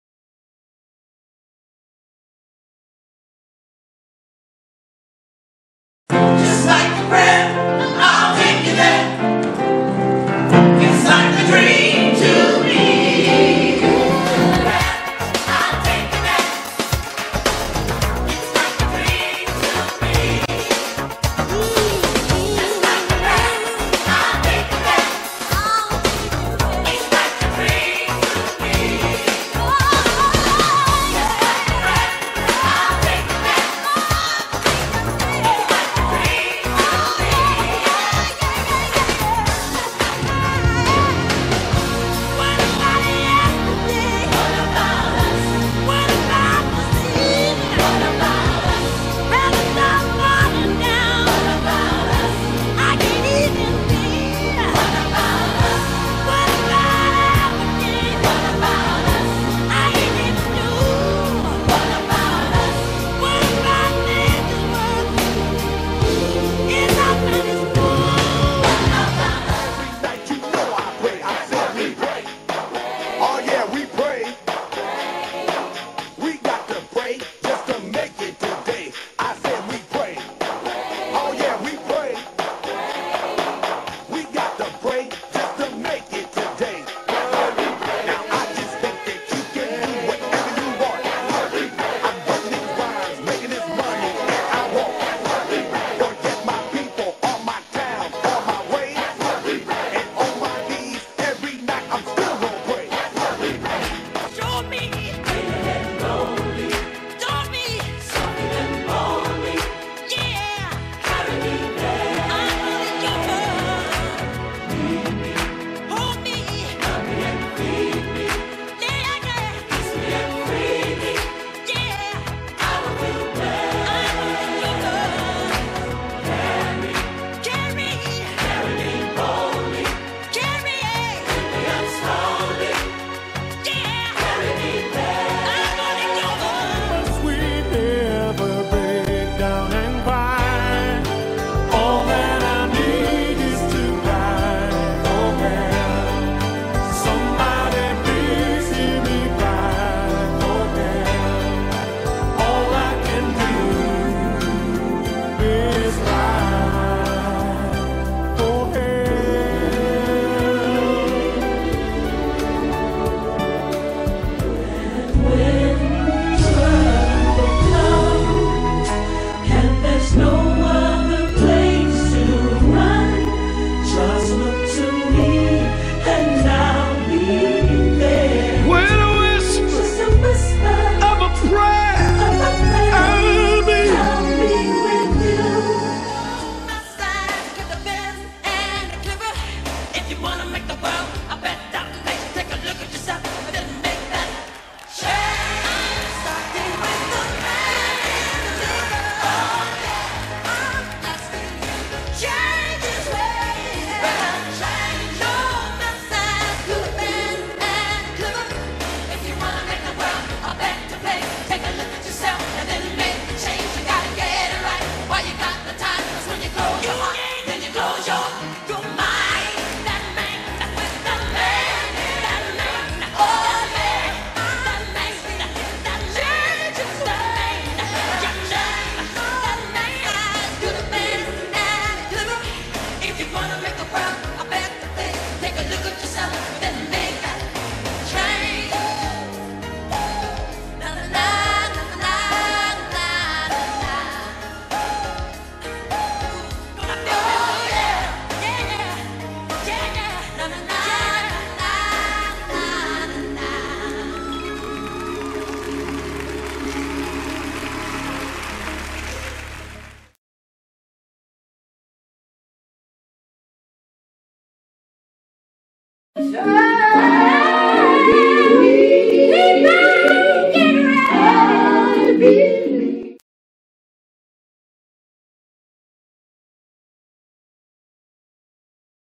Geary Faggett Ensemble Reunion Concert 8.15.13 (Promo)
PH Balanced Music presents the Geary Faggett Ensemble Reunion Concert at H.O.M.E. 90210 Supper Club in Beverly Hills. Featuring the original members of the Geary Faggett Ensemble with special guests.
No ownership of videos. (PROMO use only)